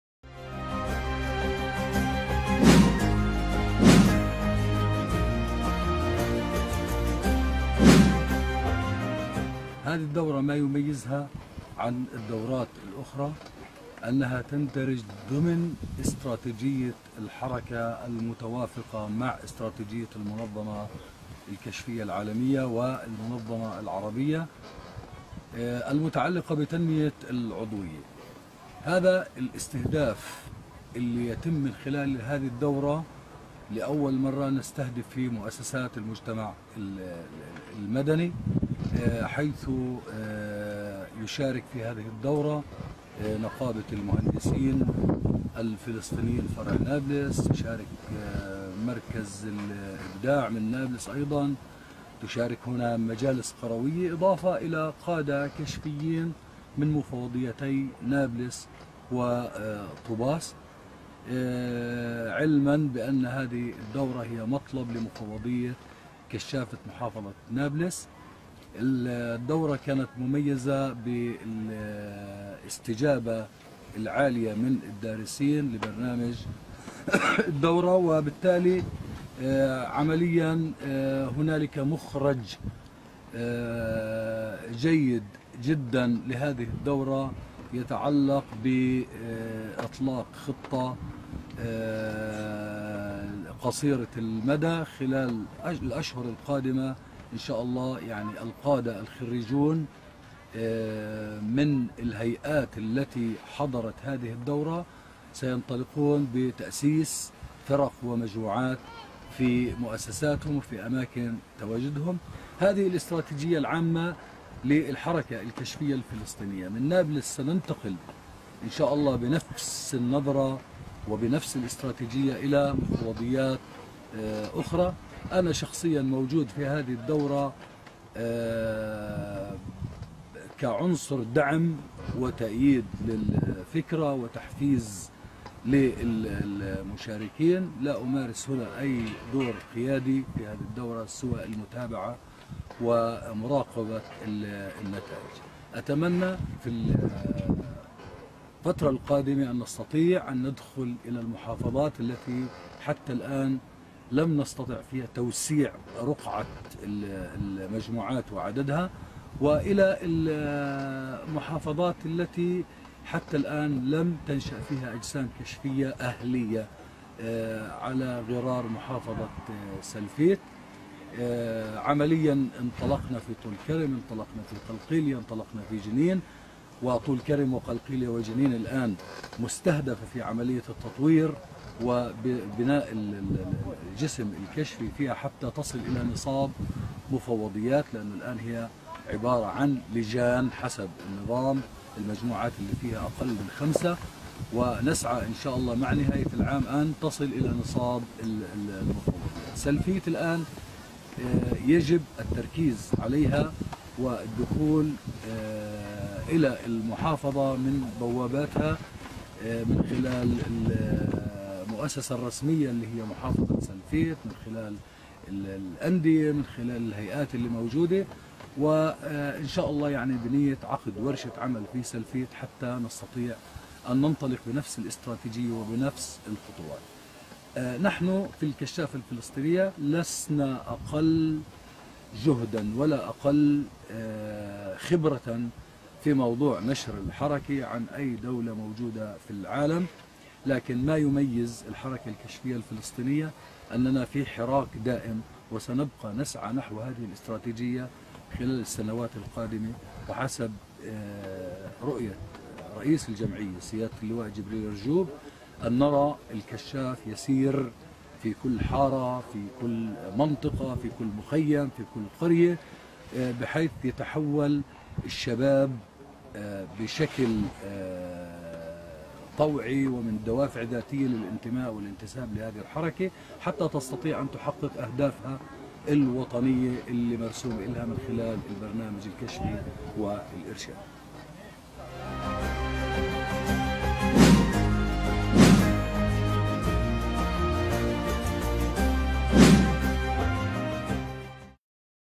القائد محمد سوالمة - يتحدث عن الدراسة الابتدائية لمفوضية نابلس 2018
إعداد - اعلام مفوضية نابلس - نائب رئيس جمعية الكشافة الفلسطينية القائد محمد سوالمة يقول عن الدراسة الابتدائية التي عقتدتها مفوضية كشافة محافظة نابلس بتاريخ 19-21 الشهر الجاري "هذه الدورة مميزة بمحتواها ومشاركيها الامر الذي يجعلها تختلف عن الدورات الاخرى، فكل ذلك يندرج ضمن استراتيجية الحركة المنبثقة عن استراتيجية المنظمة الكشفية العالميه والمنظمة العربية المتعلقة بتنمية العضوية".